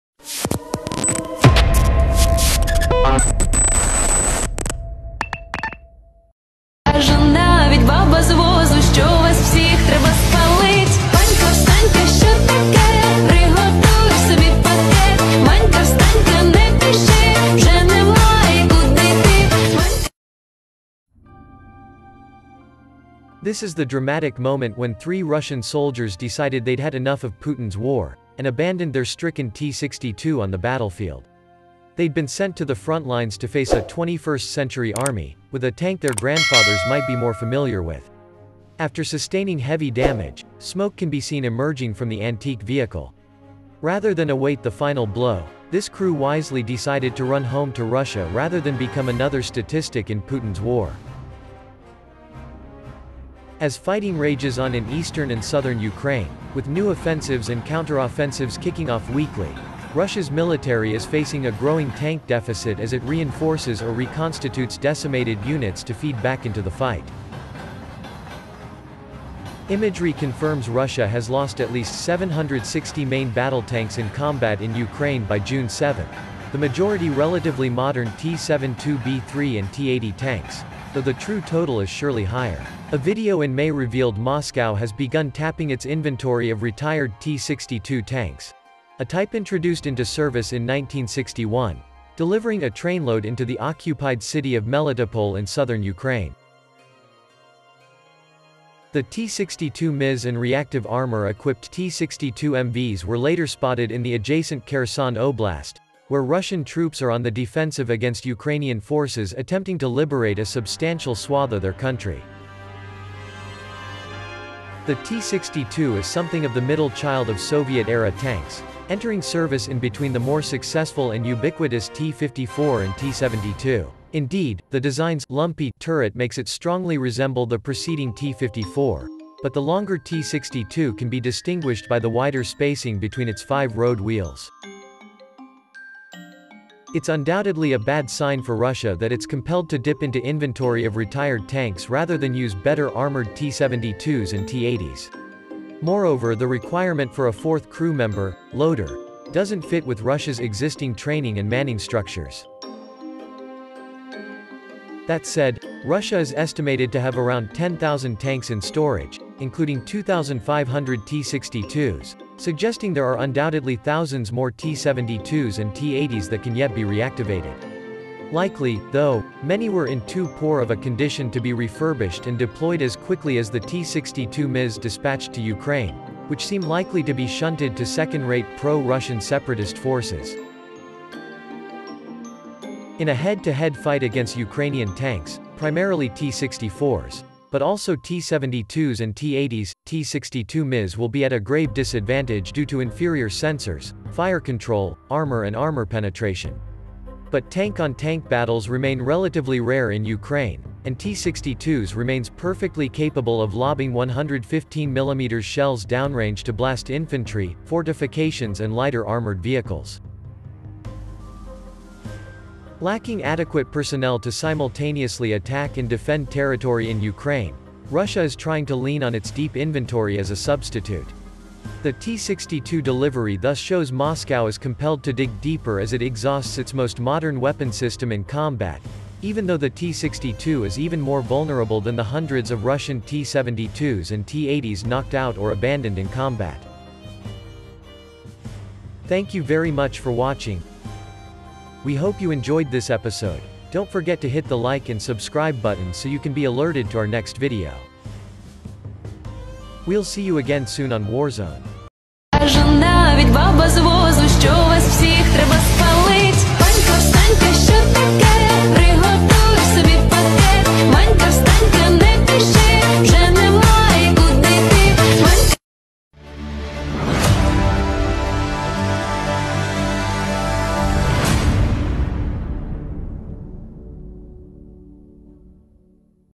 Three Russian’s in an Ancient T62 Tank have had enough of Putin’s War
Much has been certain written so far in this war about Russia's lack of modern advanced equipment. One such example of the lengths they're having to go to in order to find military equipment, has been in the various sightings of Soviet-era T62 tanks. In this video, we witness three young Russian soldiers barely escape with their lives. They managed to find a way out of a vehicle that their grandfathers might be more familiar with. This video shows them fleeing across the open fields to safety...